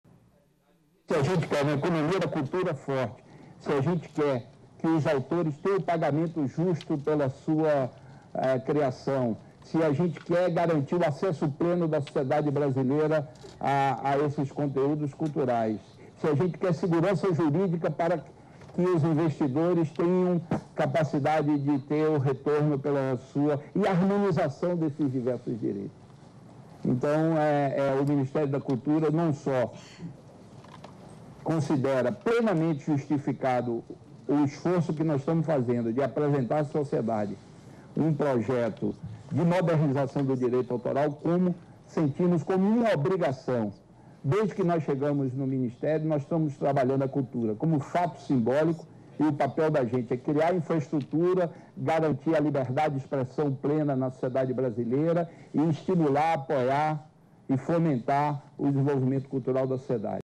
MinC lança consulta pública sobre reforma da lei de direitos autorais
Assuntos como cópia privada, uso educacional de obras protegidas e  proteção ao autor fazem parte de anteprojeto da reforma da lei de direitos autorais. O Ministério da Cultura quer ouvir o brasileiro e colocou a proposta sob consulta pública. O objetivo é harmonizar a proteção aos direitos do autor, o acesso do cidadão ao conhecimento e a segurança jurídica ao investidor.